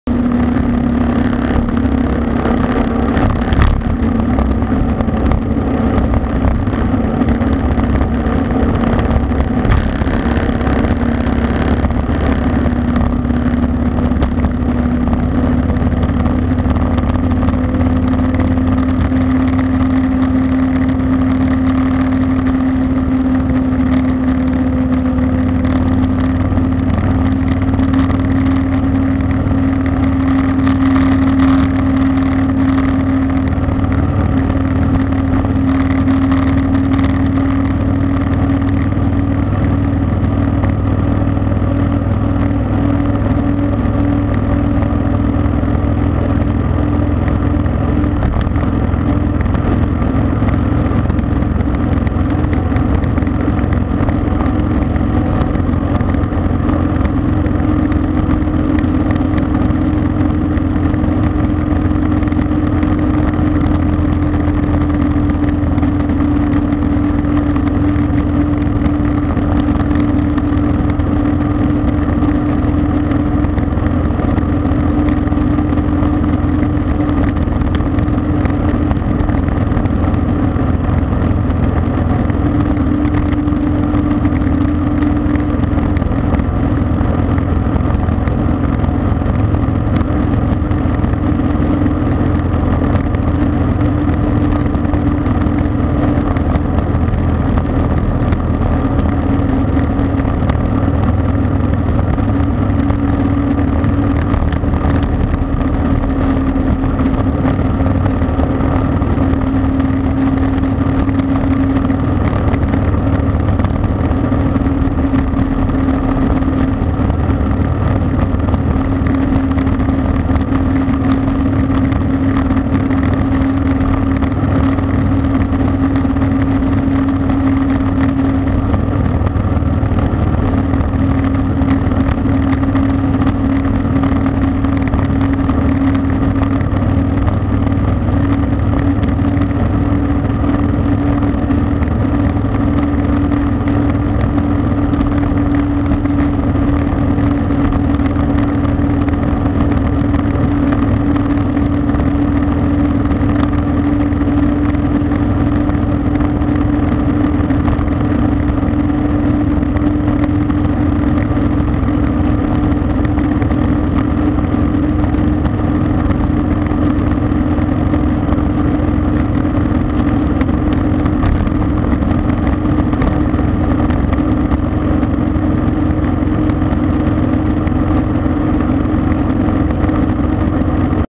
DO NOT pack your drinks on motorcycles for the road like this
My coffee loving friend fired off two Java Monster torpedoes. One I caught on video, but I have no idea where the first one went. He had two onboard allegedly strapped in when we put kickstands up, but only one was caught on video firing off the back